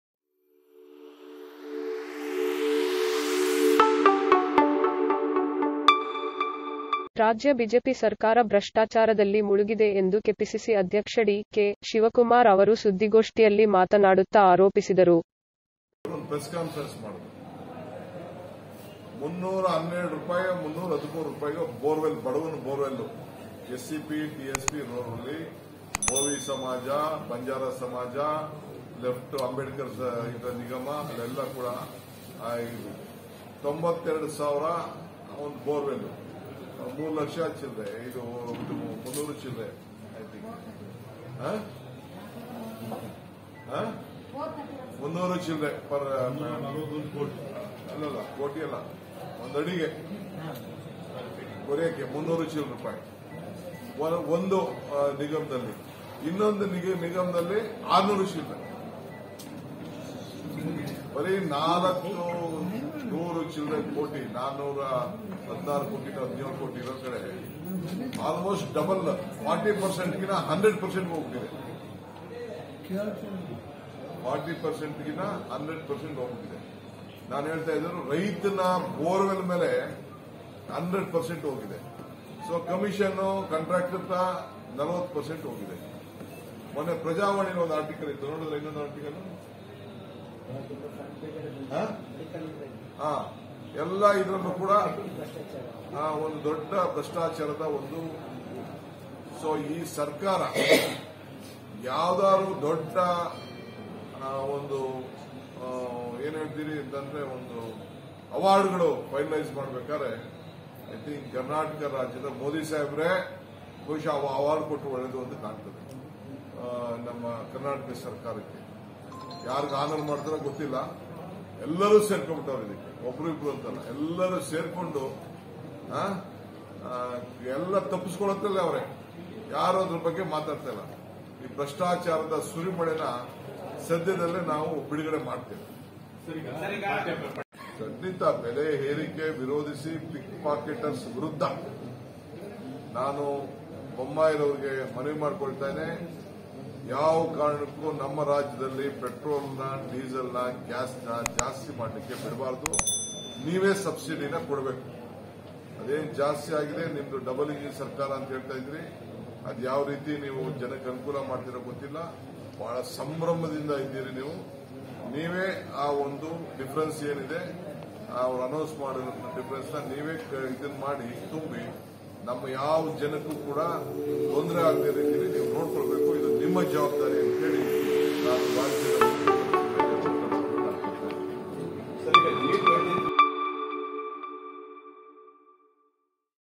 ಬಿಜೆಪಿ ಸರ್ಕಾರ ಭ್ರಷ್ಟಾಚಾರದಲ್ಲಿ ಮುಳುಗಿದೆ: ಡಿಕೆಶಿ
ಕಲಬುರಗಿ: ರಾಜ್ಯ ಬಿಜೆಪಿ ಸರ್ಕಾರ ಭ್ರಷ್ಟಾಚಾರದಲ್ಲಿ ಮುಳುಗಿದೆ ಎಂದು ಕೆಪಿಸಿಸಿ ಅಧ್ಯಕ್ಷ ಡಿ.ಕೆ.ಶಿವಕುಮಾರ್ ಅವರು ಸುದ್ದಿಗೋಷ್ಠಿಯಲ್ಲಿ ಮಾತನಾಡುತ್ತ ಆರೋಪಿಸಿದರು.